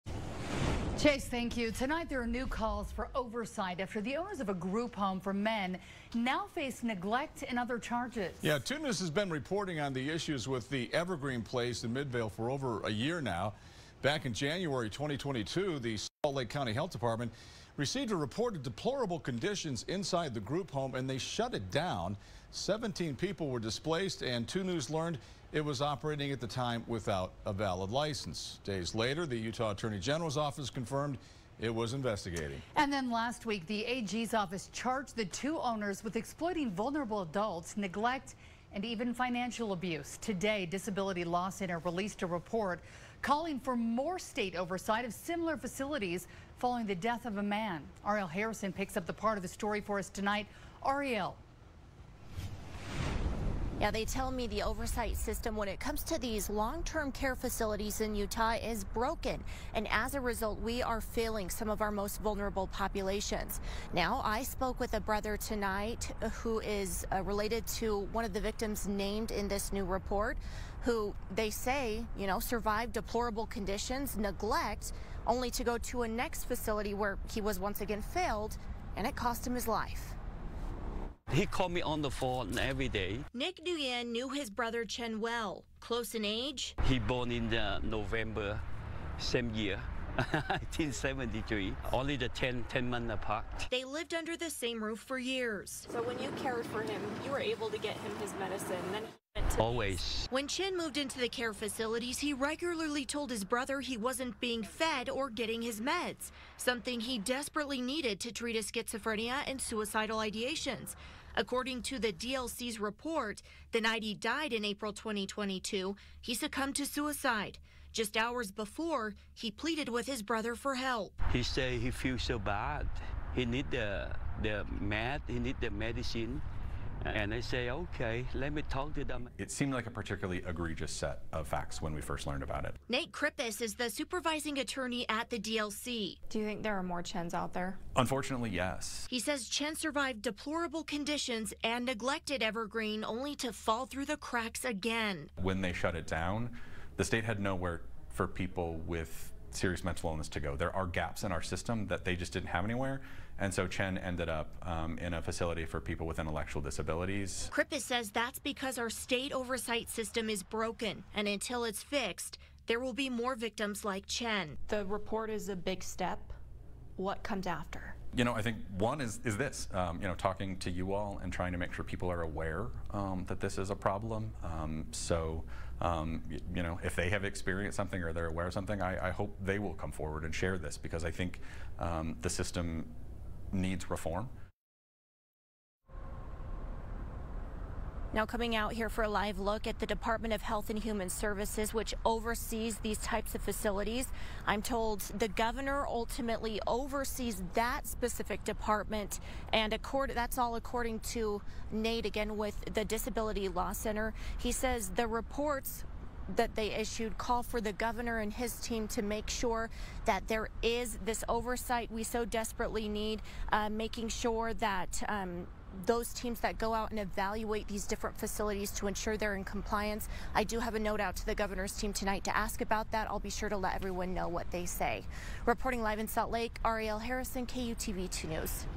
Utah long term care facilities face renewed calls for state oversight after resident death
MIDVALE, Utah (KUTV) — The Disability Law Center issued a new report highlighting the need for increased state oversight of long-term care facilities to protect some of the most vulnerable populations following the death of one resident.

The renewed calls come as the owners of a group home for men are now facing charges of neglect and exploitation.

2News has been reporting on the issues with "Evergreen Place" in Midvale for over a year now.